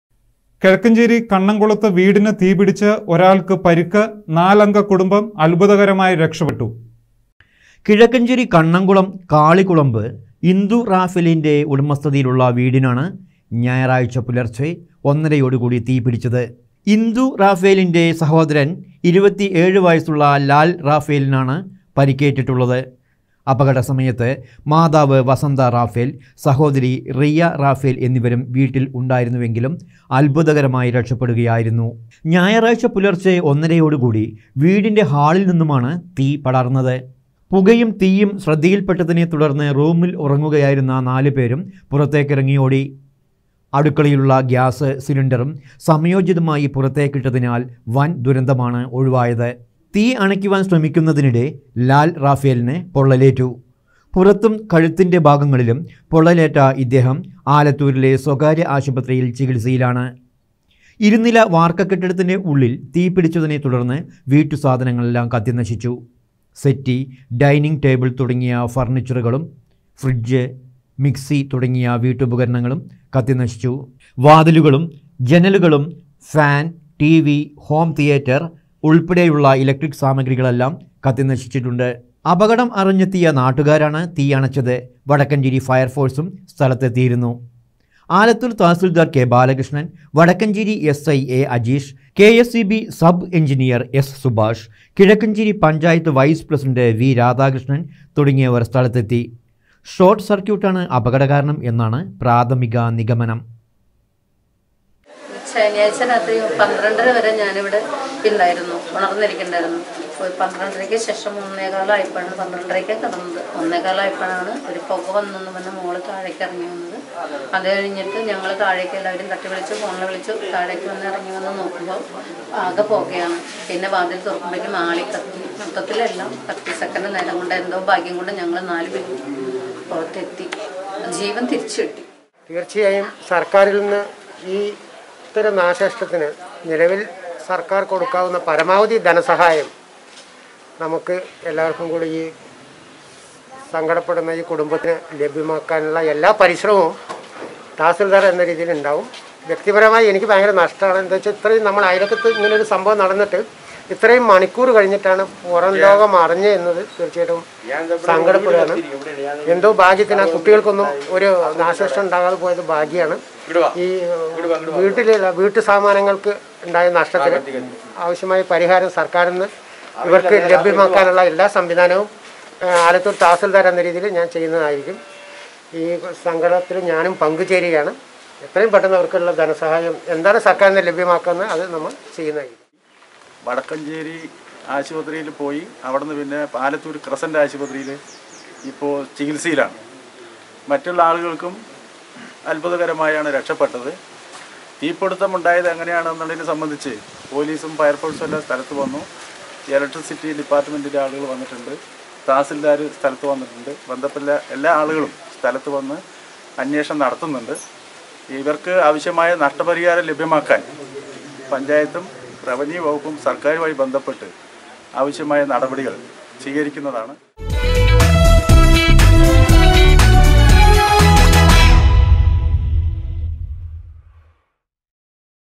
കണ്ണംകുളത്ത് വീടിന് തീപിടിച്ച് ഒരാൾക്ക് പരിക്ക്. നാലംഗ കുടുംബം അത്ഭുതകരമായി രക്ഷപ്പെട്ടു.
കിഴക്കഞ്ചേരി കണ്ണംകുളത്ത് വീടിന് തീപിടിച്ച് ഒരാൾക്ക് പരിക്ക്. നാലംഗ കുടുംബം അത്ഭുതകരമായി രക്ഷപ്പെട്ടു.